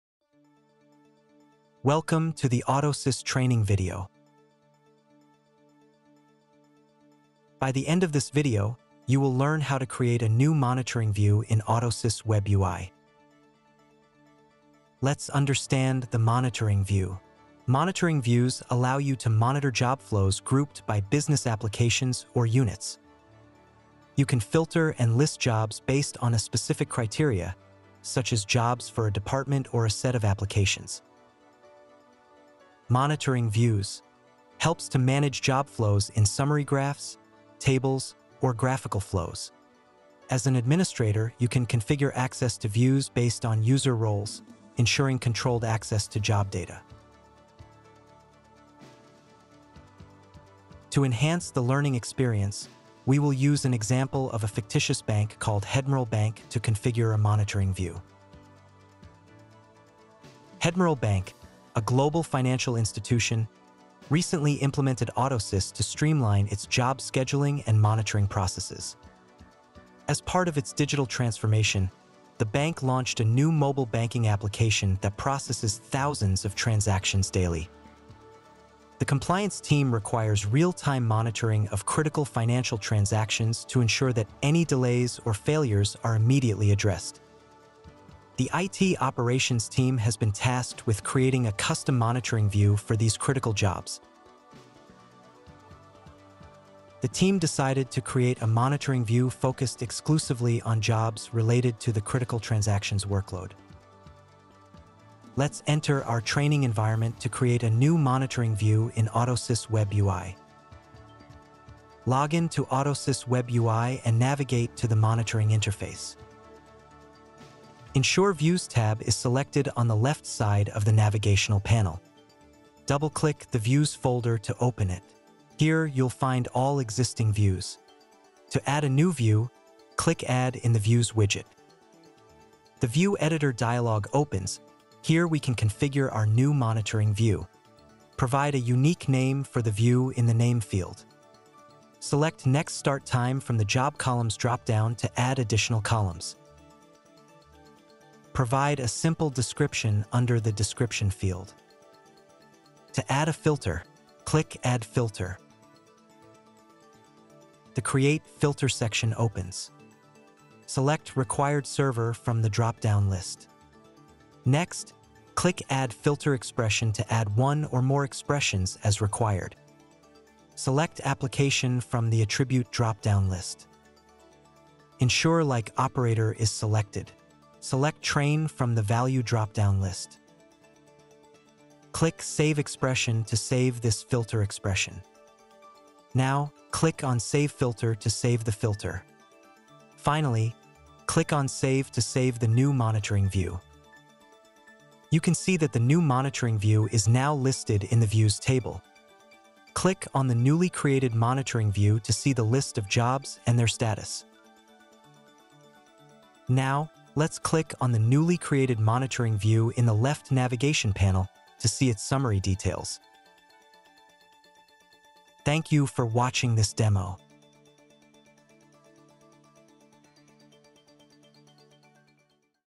AutoSys Web UI: Create New Monitoring View
In this video, you will learn how to create a new Monitoring View in AutoSys Web UI to effectively monitor job flows and critical workloads in your environment.

What is a Monitoring View?

Monitoring Views in AutoSys allow you to:
🔹 Group jobs by specific business applications or departments.
🔹 Monitor job flows through summary graphs, tables, and visual flows.
🔹 Apply filters to display only relevant job data.
🔹 Control access to views based on user roles for enhanced security.

Whether you are a beginner looking to master the basics of AutoSys or an experienced user seeking advanced insights, this tutorial has something for you. By the end of this video, you will be equipped to create a new Monitoring View in AutoSys Web UI.